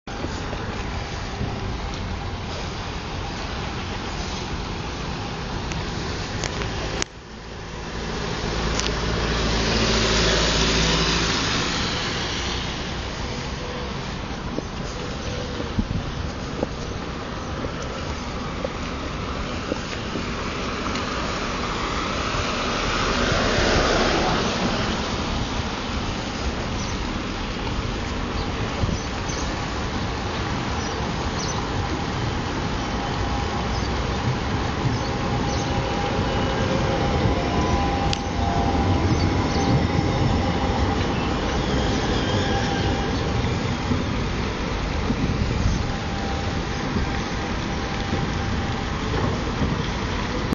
Linea 4 MetroValencia Rotonda Pont de Fusta
Tranvía de la Linea 4 de MetroValencia haciendo la rotonda de Pont de Fusta 17/4/2010 14:05 del mediodia